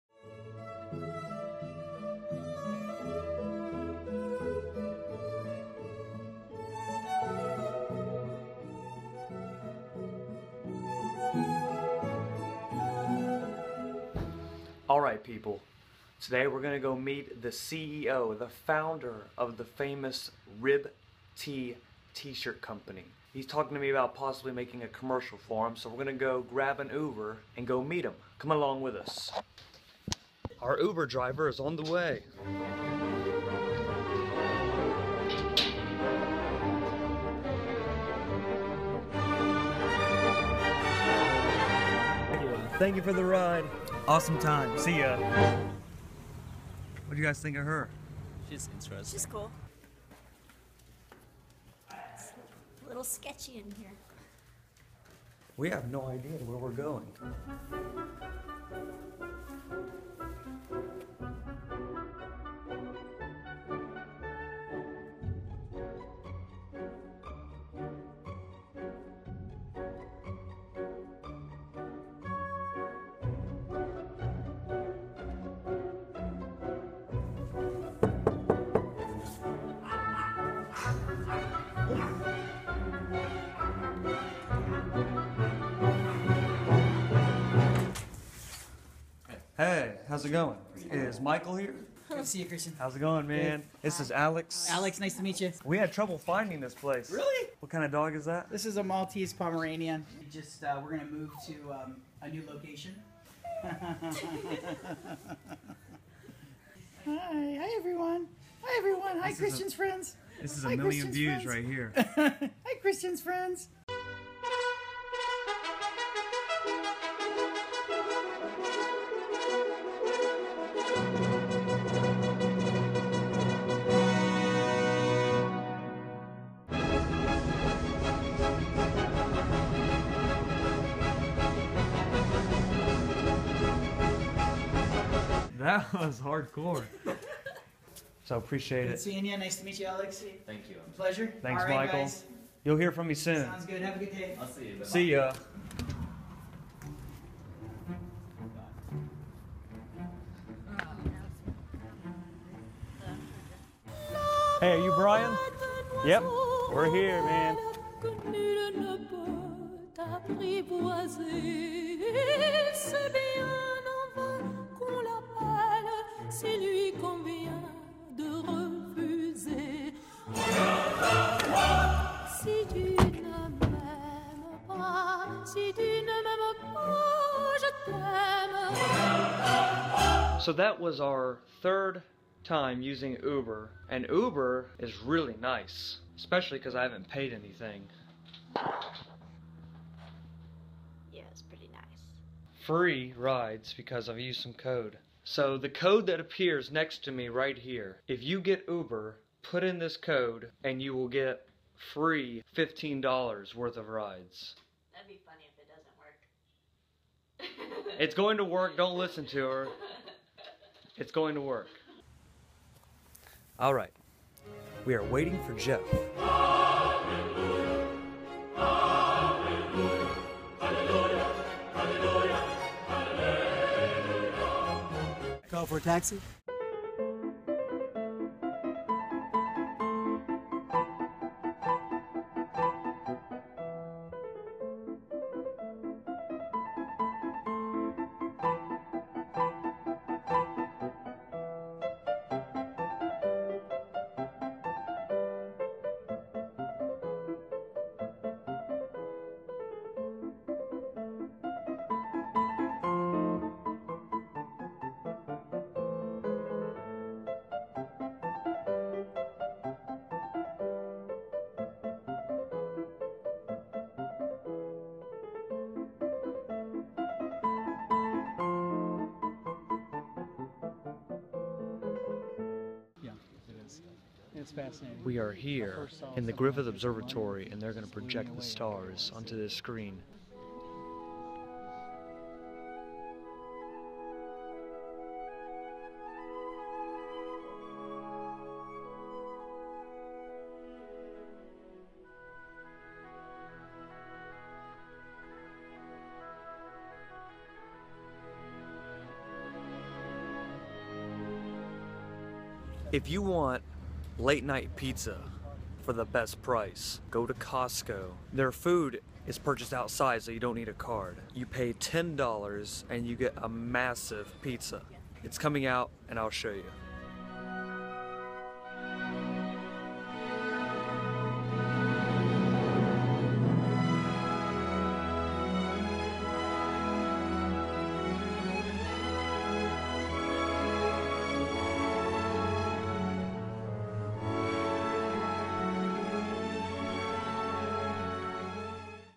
Day 7 in LA - Super Horny Dog
Last day in LA and last vlog! Thanks for joining me on the little adventure!